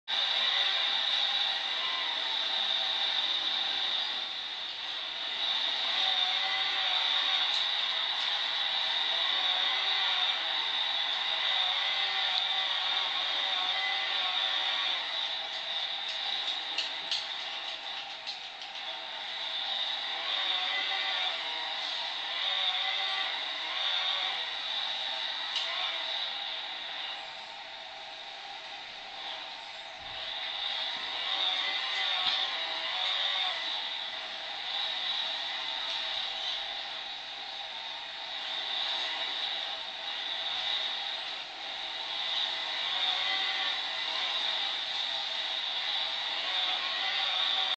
Rovin - Analog Chair
Rovin sitting in a chair that turns automatically.^^